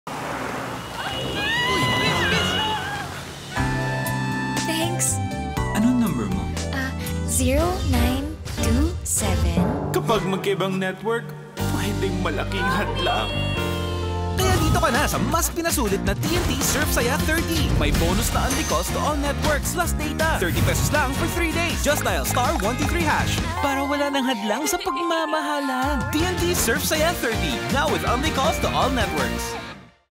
Ang TNT SurfSaya 30, may bonus unli allnet calls na!
Wala nang hadlang sa pagmamahalan, magkaibang network 'man. Get TNT SurfSaya 30 with BONUS unlicalls to all networks! May 300MB data + 100MB daily FB & Messenger access ka pa! Dial *123# to register.

Available until September 30 only! Please expect your BONUS UNLI ALLNET CALLS within 24 hours upon successful availment.